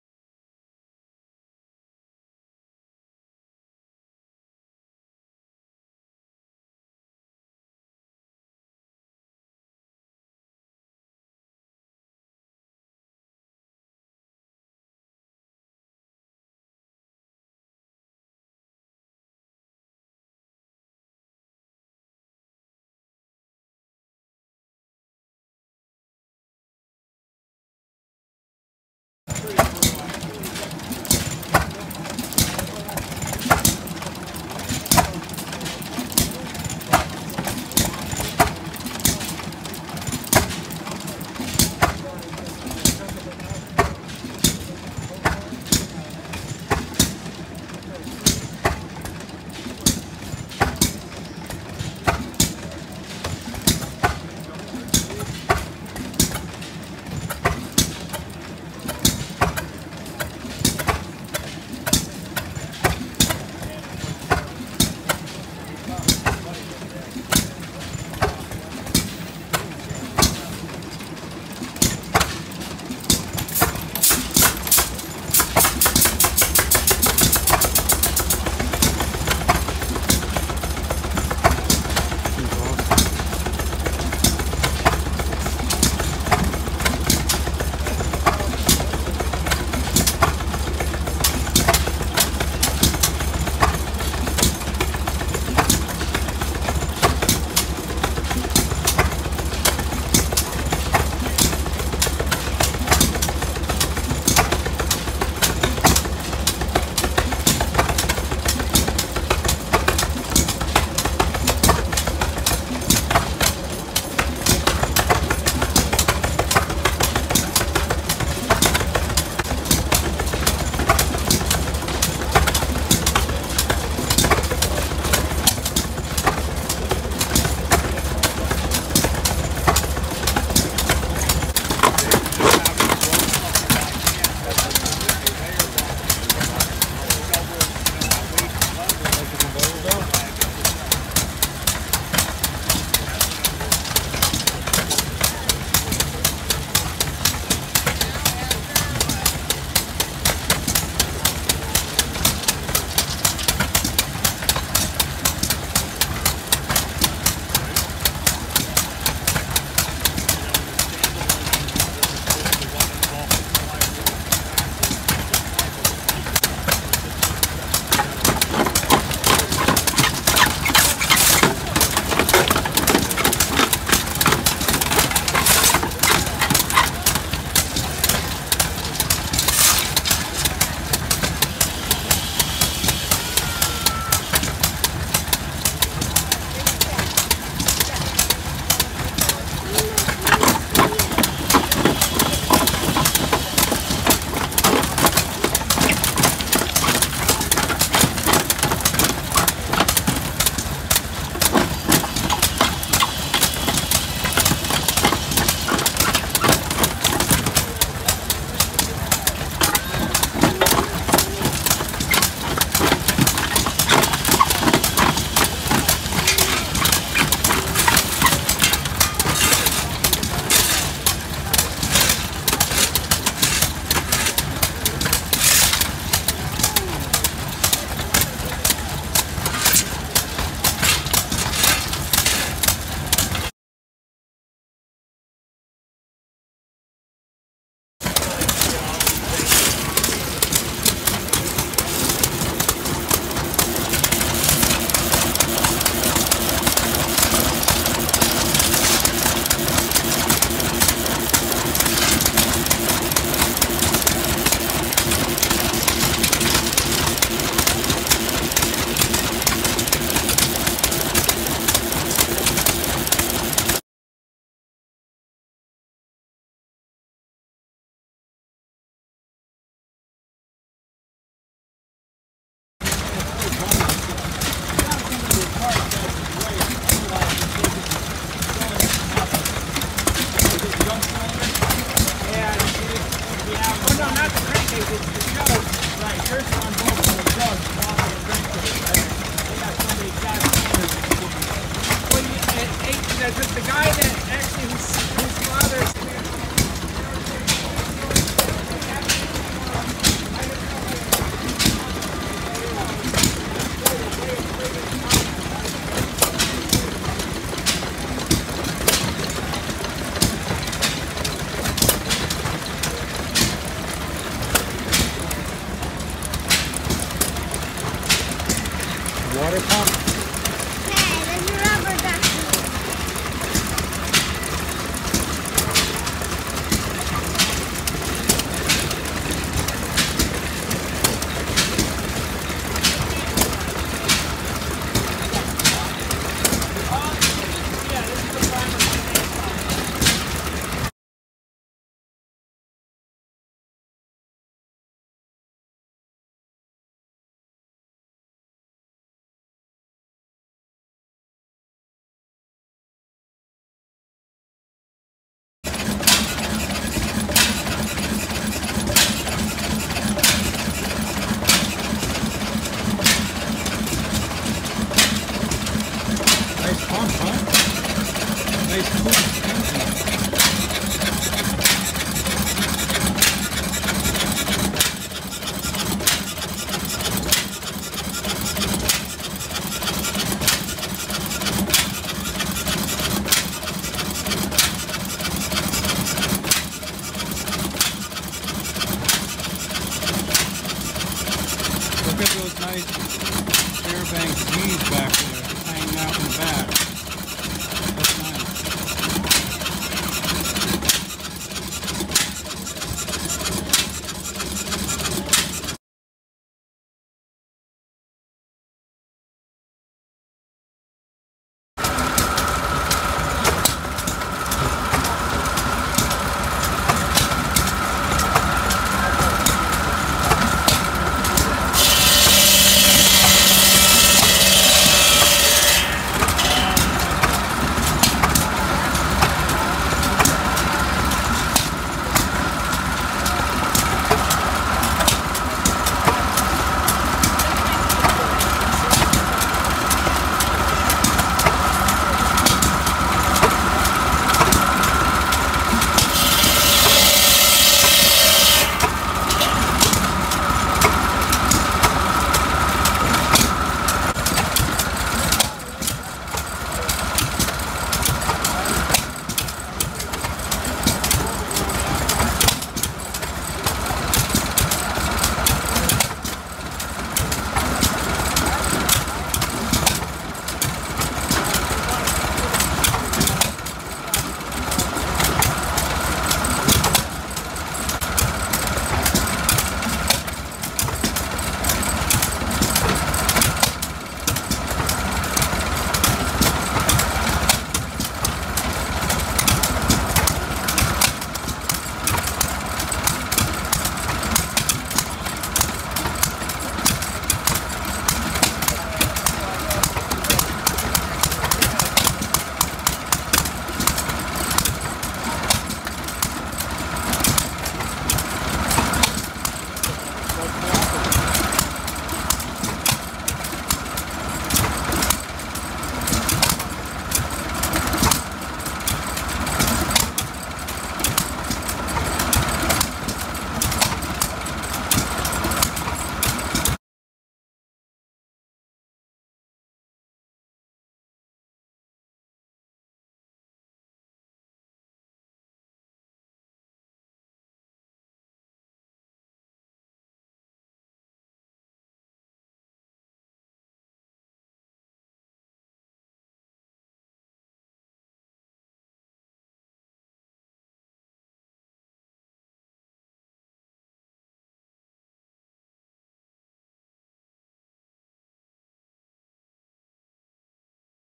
Dutchess County Fair Engine Demonstration 2021 Hit and Miss
Dutchess County Fair (New York) Engine Demonstration 2021.  The Boice family does a first class job showing a bunch of engines with local roots.  I have been going to the fair since I was a little kid and have seen this display for years but it never gets old……

James Boice Sr (deceased) was an avid Gravely Collector, he worked for a Gravely dealer and after he passed I was able to browse his collection.  He had some pretty rare Gravely’s including a Jenkins Head and a hex axle…